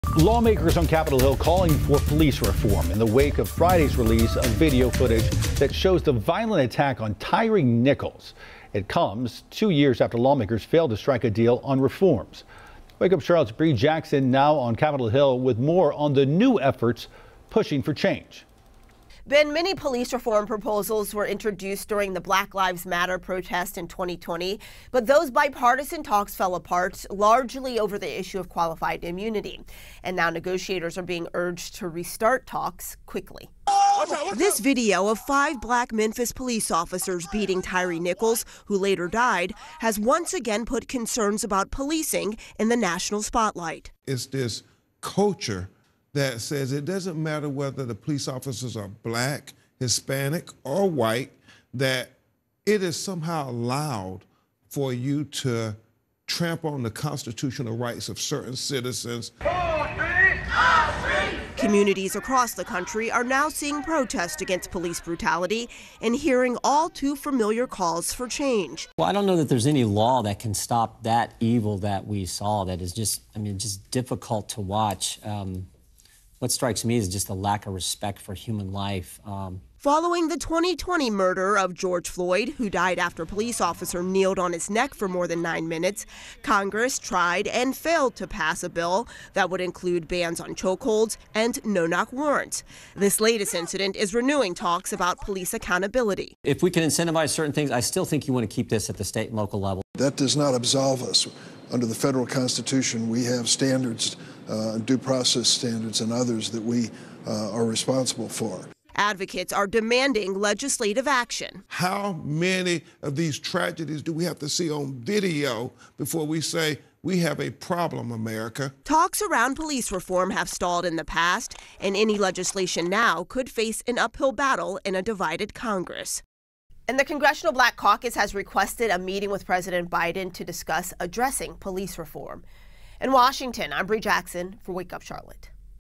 US lawmakers call for police reform after Tyre Nichols video release
Memphis Police announced Saturday they are permanently disbanding the SCORPION unit, which the five MPD officers charged with Tyre Nichols' death were assigned to.

MPD said Chief C.J. Davis met with officers assigned to the SCORPION unit, and they agreed "unreservedly" with disbanding the unit, and said more needed to be done to help heal the community.

Nichols died three days after the Jan. 7 encounter with officers from the SCORPION unit.

Five officers have been charged with second degree murder, aggravated kidnapping, aggravated assault, official misconduct, and official oppression in his death.